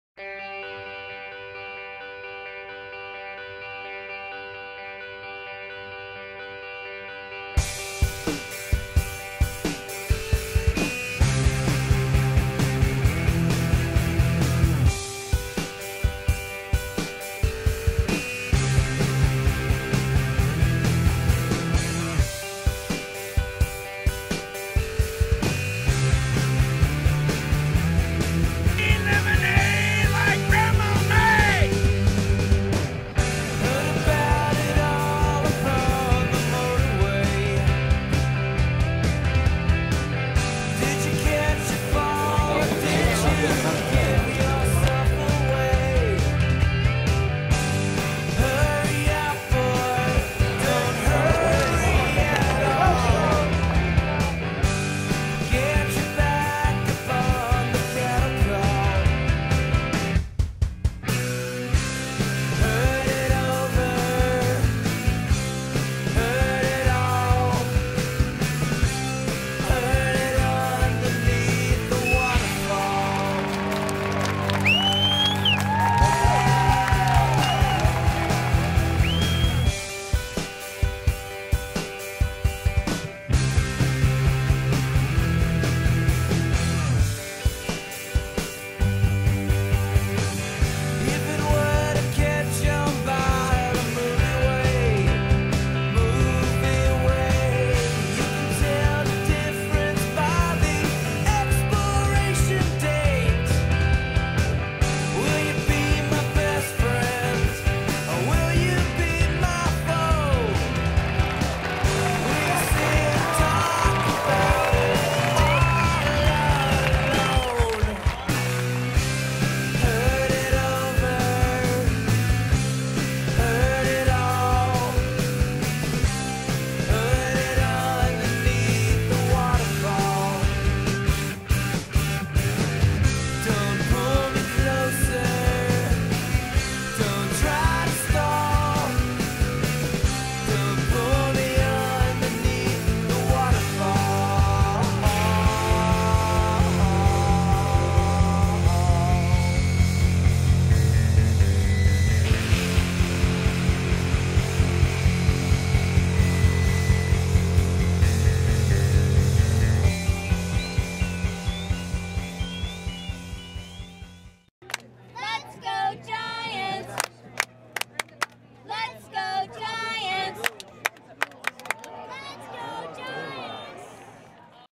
San Francisco Giants Spring Training 2012
A quick look at the San Francisco Giants during spring training in Arizona this year. Shot at Scottsdale Stadium and the Peoria Sports Complex.